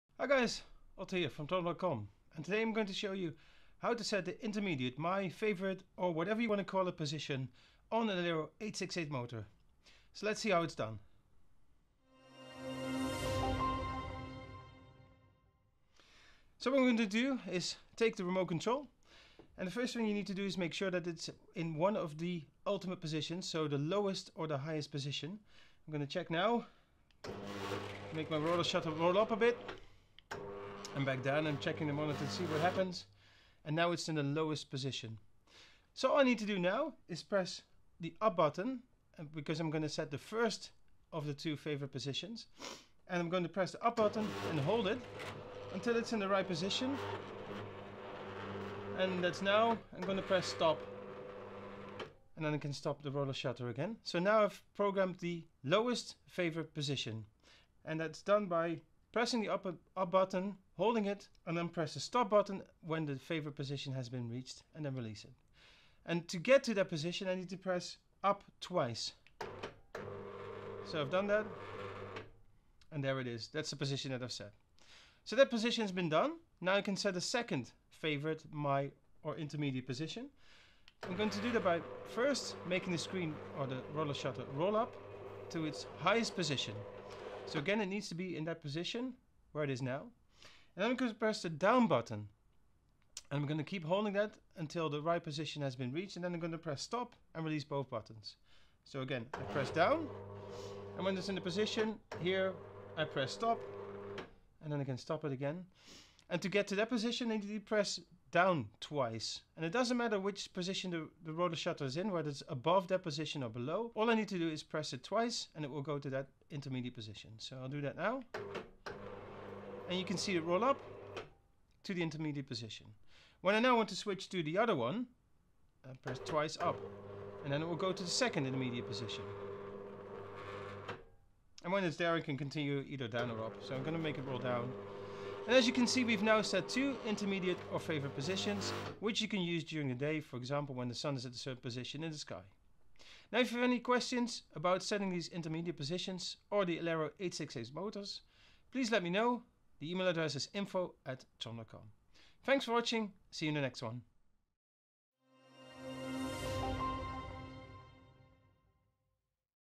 How to set 2 favourite intermediate positions for an Elero 868 remote controlled motor
In this video, we show you how to set 2 seperate favourite positions for your Elero 868 remote controlled motor.